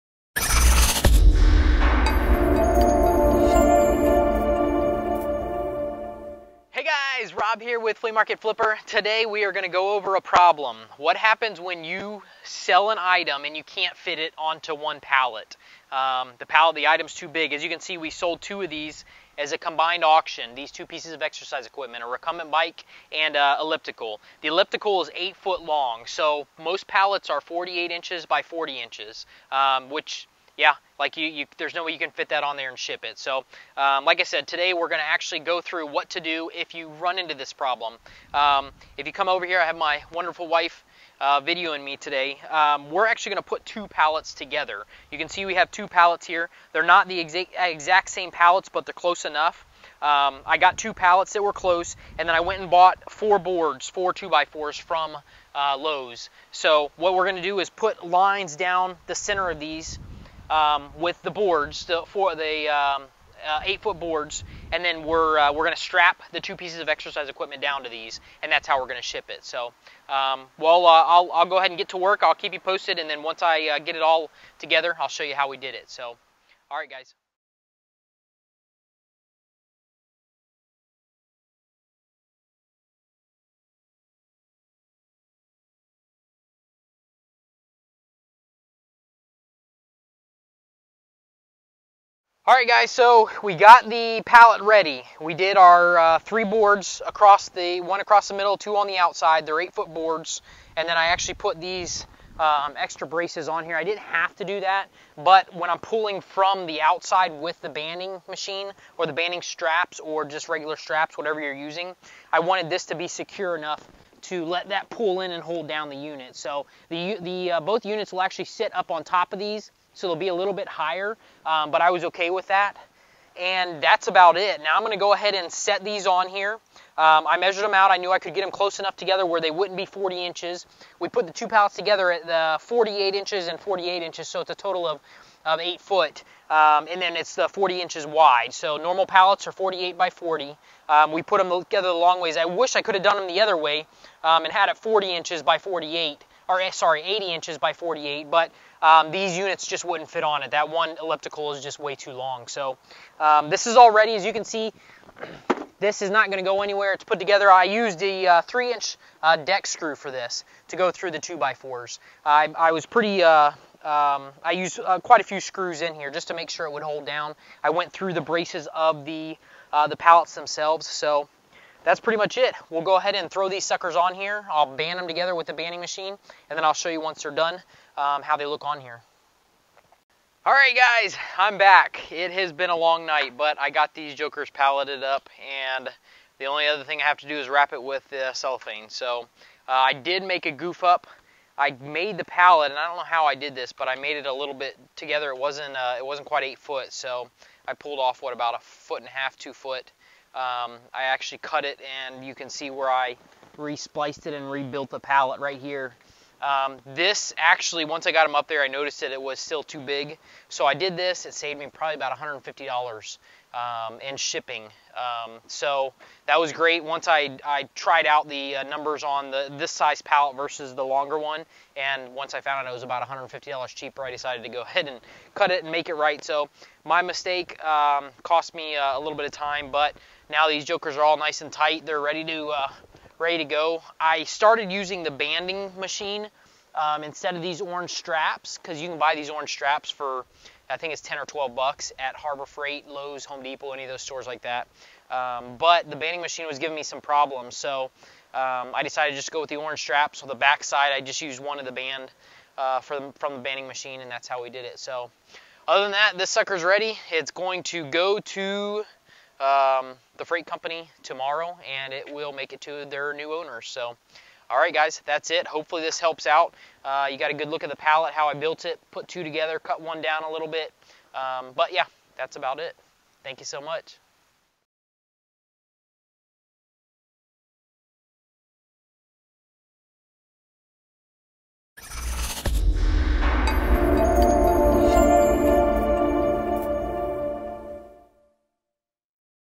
How to pallet an item that's too large for a standard pallet
How to pallet an item for shipping that's larger than a standard size pallet.